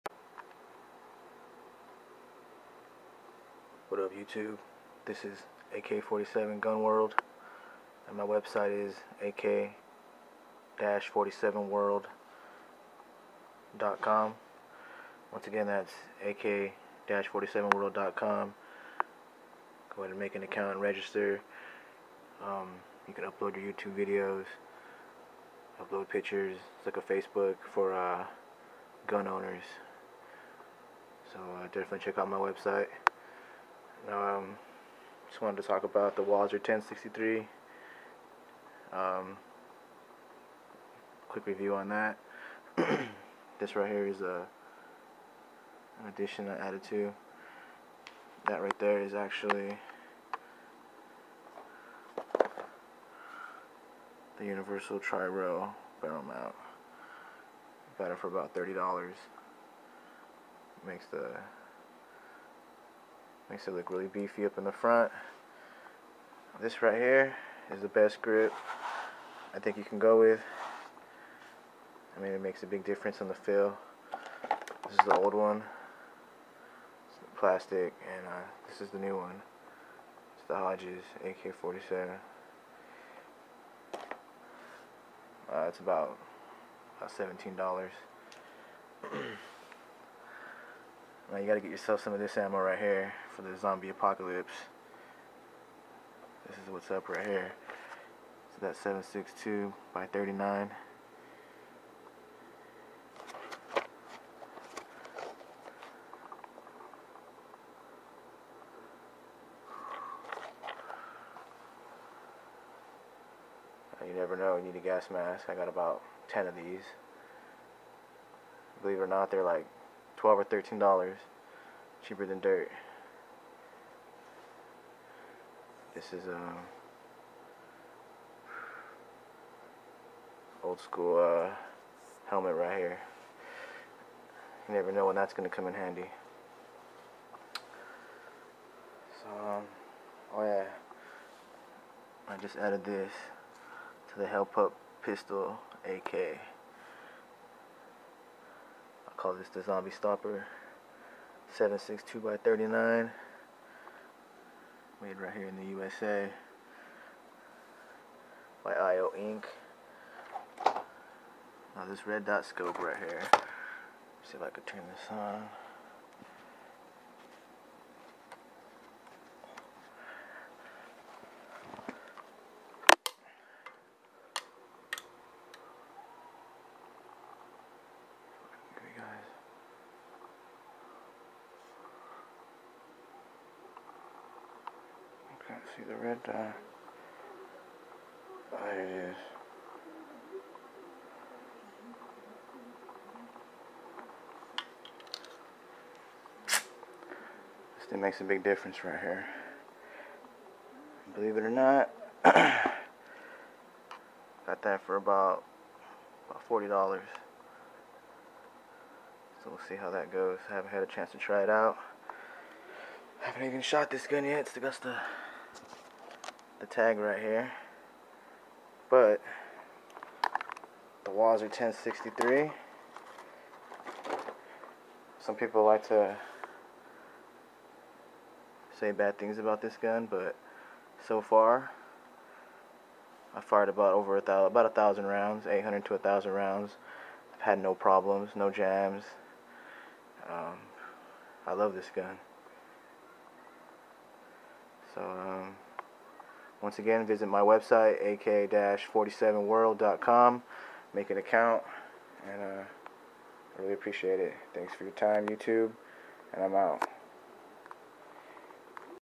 AK-47 Accessories update
Must have AK-47 items for under $40 dollars.